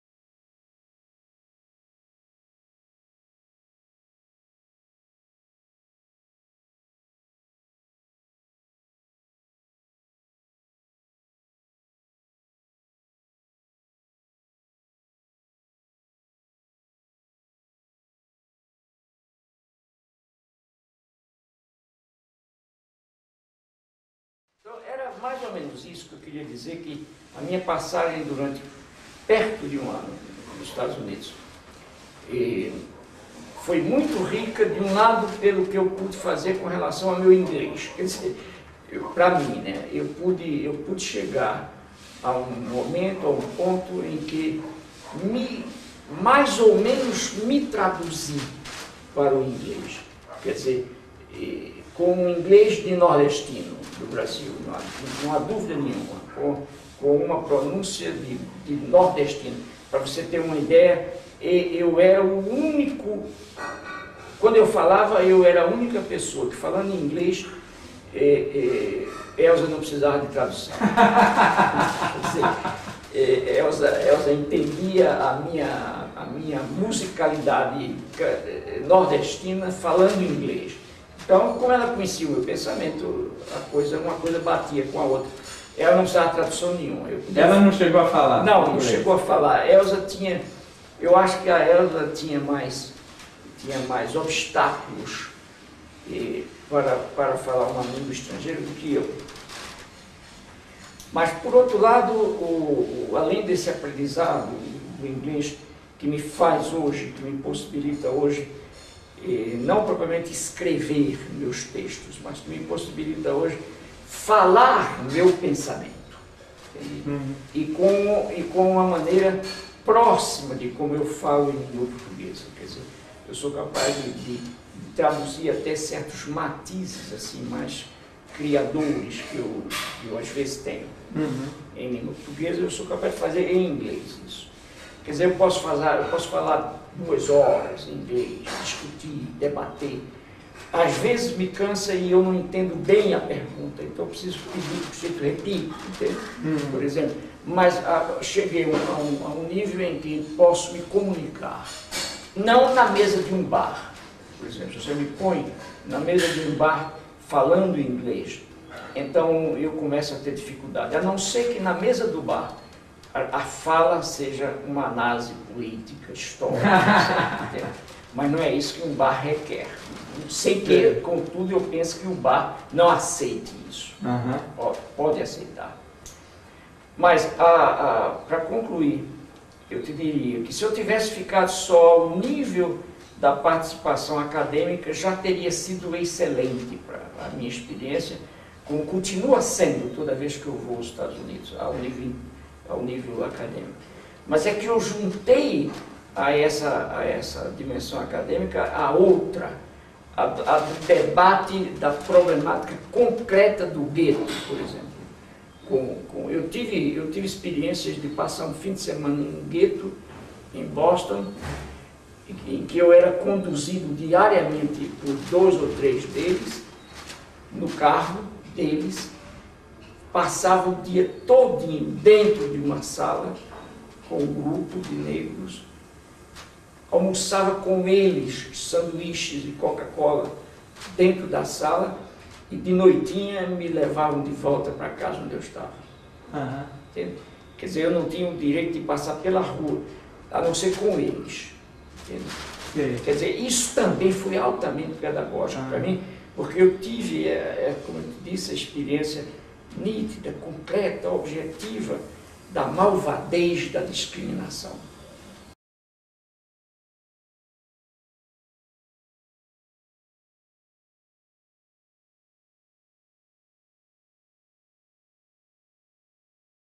L4-18 - O inglês de nordestino e "a malvadez da discriminação", no gueto negro de Boston
Em menos de um ano nos Estados Unidos, Paulo Freire já falava a língua da terra, mas com forte sotaque do nordeste brasileiro. Isso lhe permitiu, diz ele, “falar meu pensamento”, mas não sustentar conversas “numa mesa de bar”, por exemplo. Além da experiência acadêmica adquirida nesse período, Paulo fala de suas experiências de fim de semana num gueto de Boston: “eu era conduzido diariamente no carro deles”, comenta, e passava o dia com um grupo de negros. “Almoçava com eles sanduíches e coca-cola, e de noitinha eles me levavam de volta pra casa onde eu estava”. Acrescentando que “isso também foi altamente pedagógico pra mim”, ele explica porquê.